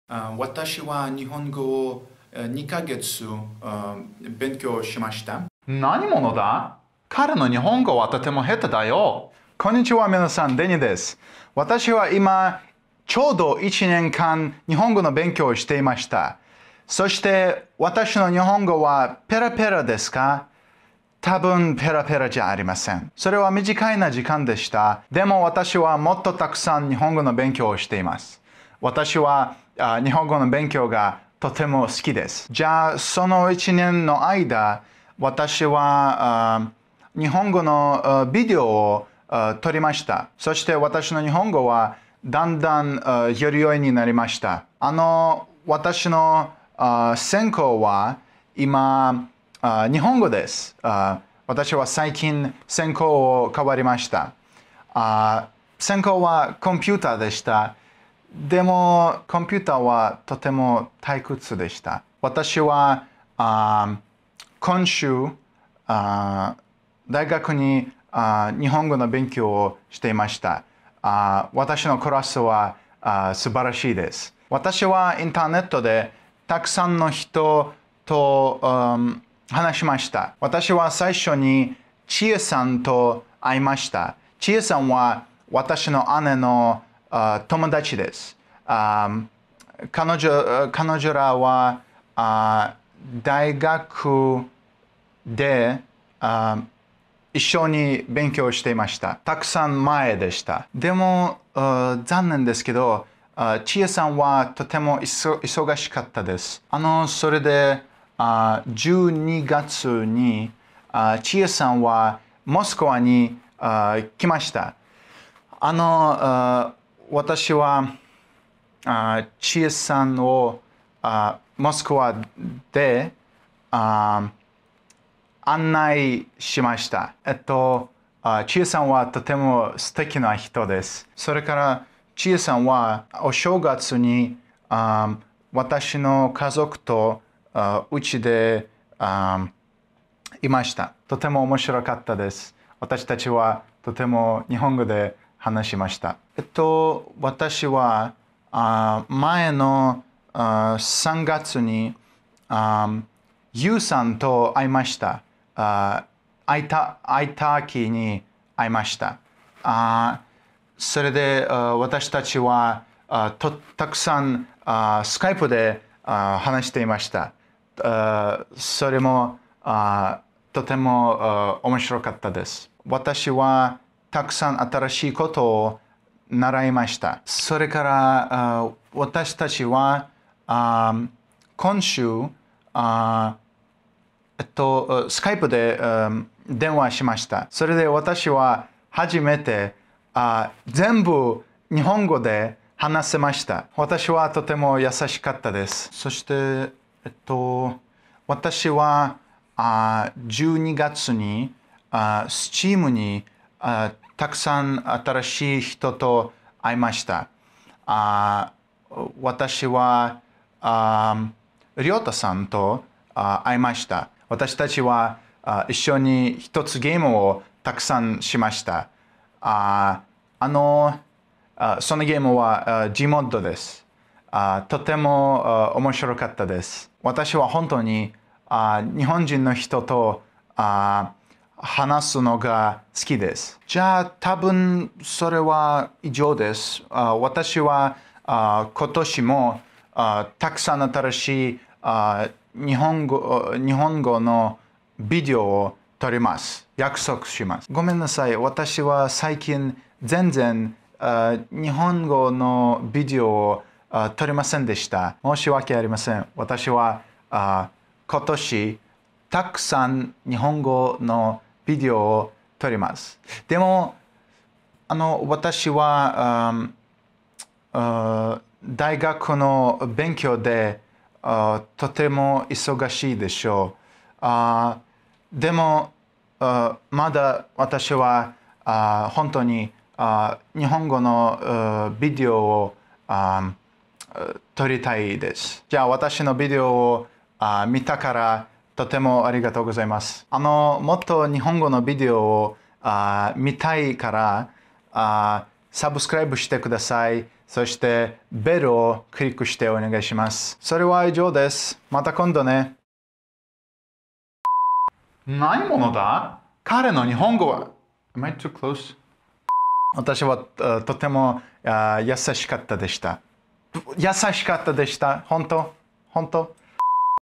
Speaking Fluent Japanese After 1 Year of Studying? (nah)
In this video I speak Japanese after having studied it by myself for exactly one year. Hope you enjoy! If you speak Japanese and find mistakes in my speech, please tell me about it in the comments.